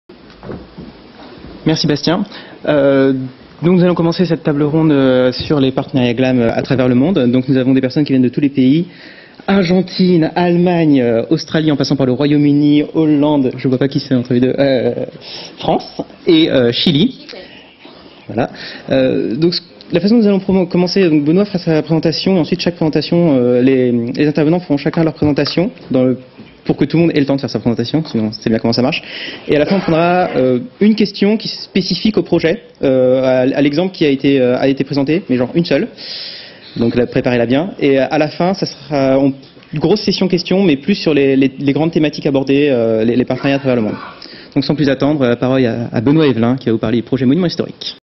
GlamWiki 2010 - Partenariats culturels - Jean-Frédéric Berthelot (Wikimédia France)
Benoît Evellin et Jean-Frédéric Berthelot animent la discussion sur les partenariats culturels avec Wikimedia dans le monde lors des Rencontres Wikimédia 2010.